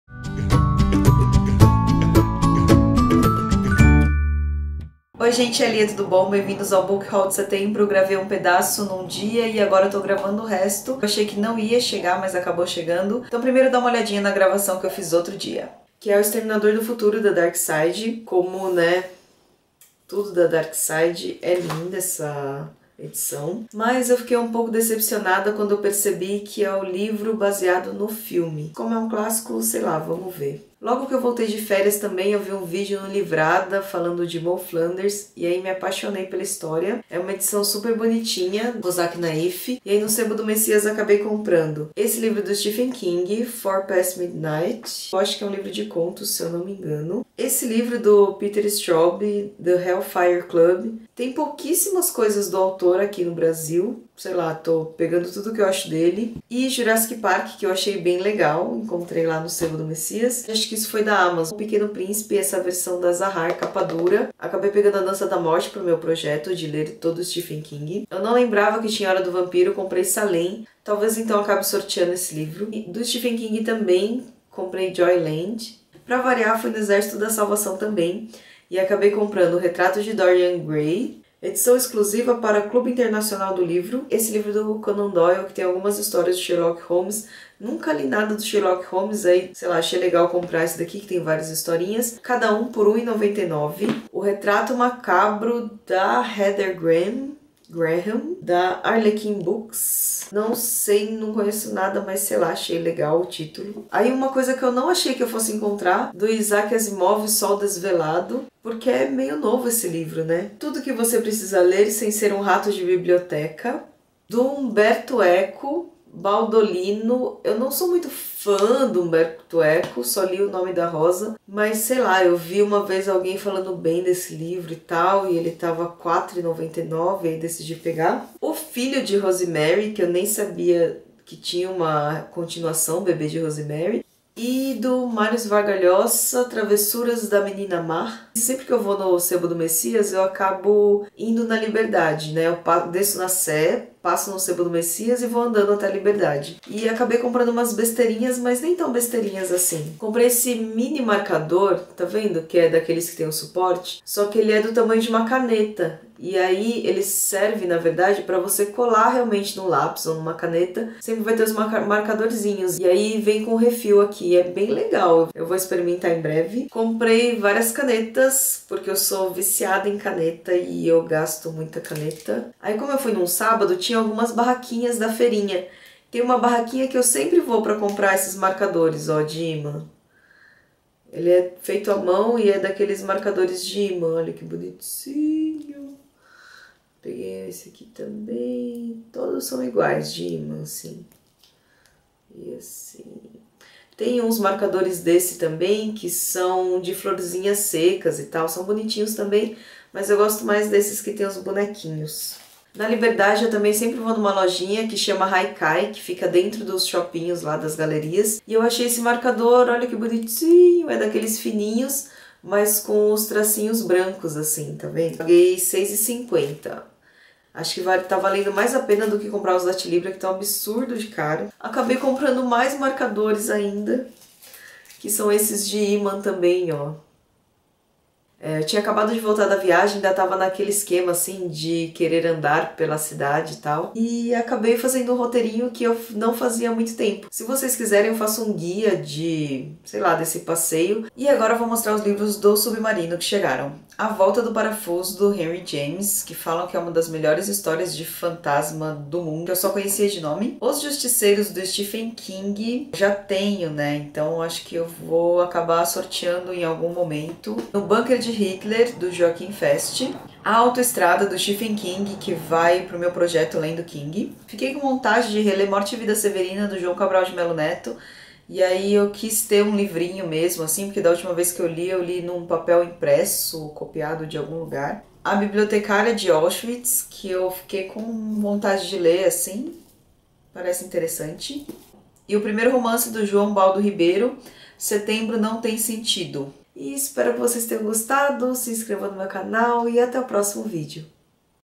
BookHaul Setembro - Frankstein e Papelaria
Uma das piores edições feitas neste canal! Mas o importante é que os livros estão aí...

- O exterminador do futuro, Bill Wisher,James Cameron,Randall Frakes - Darkside books
- Moll Flanders, Daniel Defoe - Cosac Naify
- Four past midnight, Stephen King
- The hellfire club, Peter Straub
- Jurassic Park, Michale Crichton
- O pequeno príncipe, Antoine de Saint Exupèry - Zahar
- A dança da morte, Stephen King - Ponto de leitura, Objetiva
- Salem', Stephen King - Ponto de leitura, Objetiva
- Joyland, Stpehen King - Suma de letras
- O retrato de Doryan Gray, Oscar Wilde
- Sherlock Holmes, Conan Doyle
- Retrato macabro, Heather Graham - Arlequim
- Sol desvelado, Isaac Asimov
- Baudolin, Umberto Eco
- O filho de Rosemary, Ira Levin
- Tudo que você precisa ler sem ser um rato de biblioteca
- A volta do parafuso, Henry James
- Os justiceiros, Stephen King - Ponto de leitura, Objetiva
- A autoestrada, Stephen King - Ponto de leitura, Objetiva
- No bunker de Hitler, Joaquim Fest - Ponto de leitura, Objetiva
- Morte e vida severina, João Cabral de Mello Neto - Ponto de leitura, Objetiva
- A bibliotecária de Auschwitz, Antonio G. Iturbe - Nova fronteira
- Setembro não tem sentido, João Ubaldo Ribeiro - Alfaguara